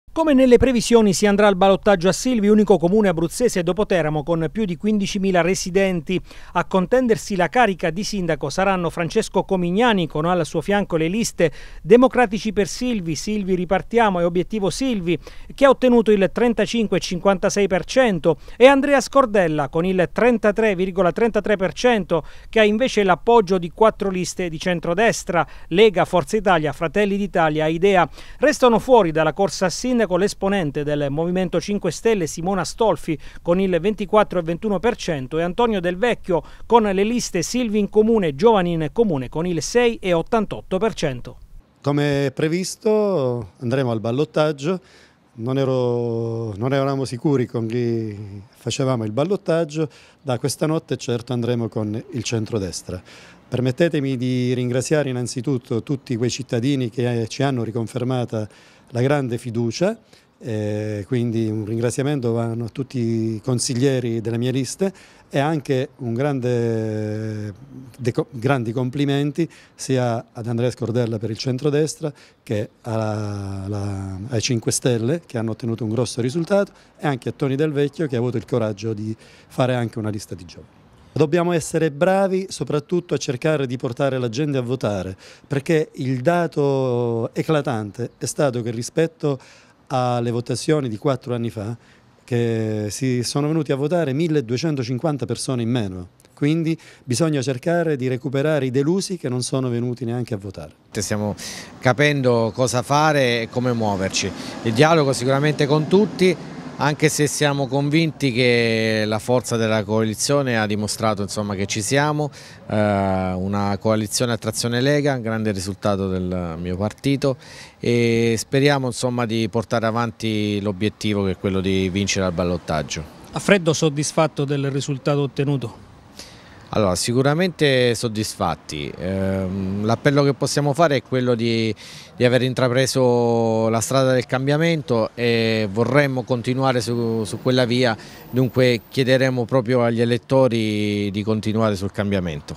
A Silvi ballottaggio Comignani Scordella
Come previsto sarà ballottaggio anche a Silvi per l'elezione del nuovo sindaco della cittadina adriatica. A sfidarsi saranno Francesco Comignani, primo cittadino prima dell'arrivo del Comissario e di area centrosinistra e Andrea Scordella, appoggiato da quattro liste di centrodestra